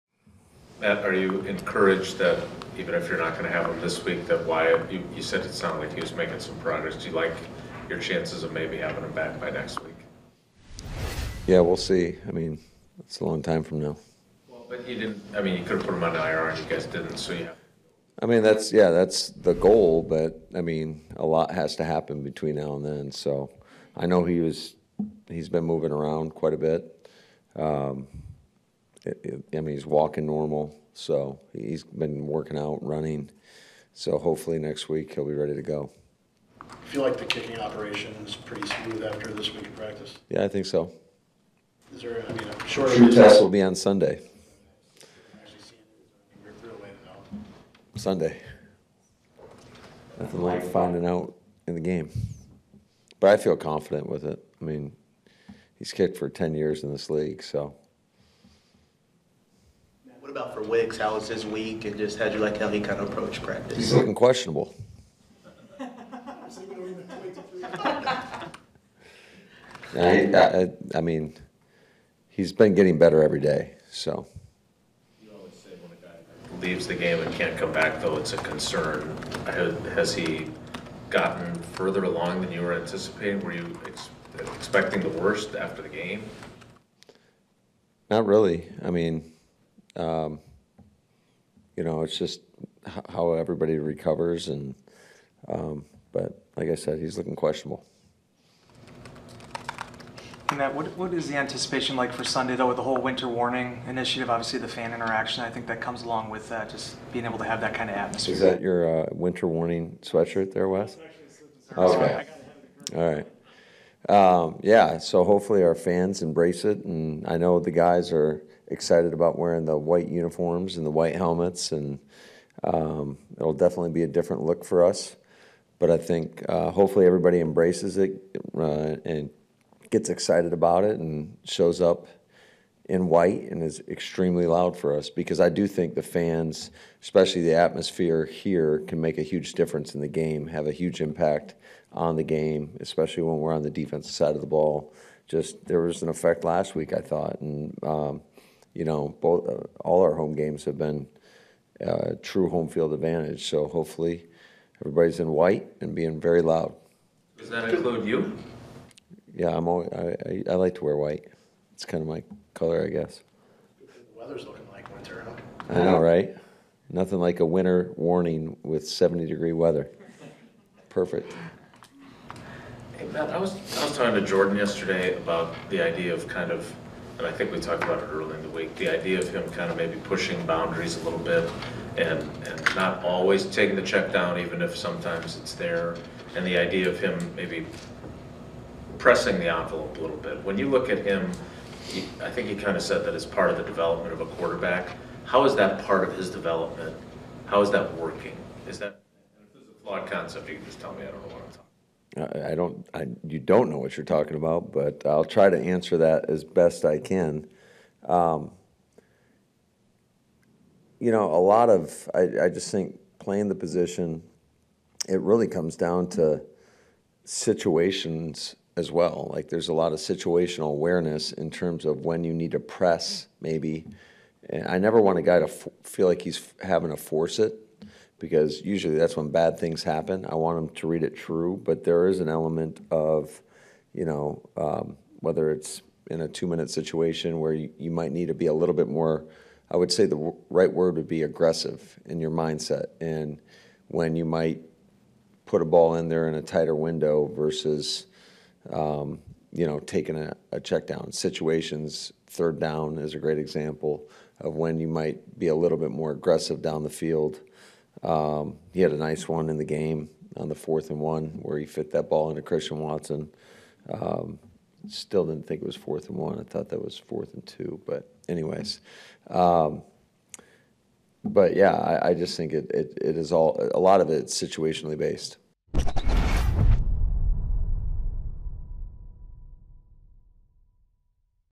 Matt LaFleur says Packers fans can ‘have a huge impact on the game’
Head Coach Matt LaFleur spoke to the media on Friday as the Green Bay Packers get set to face the Houston Texans in Week 7.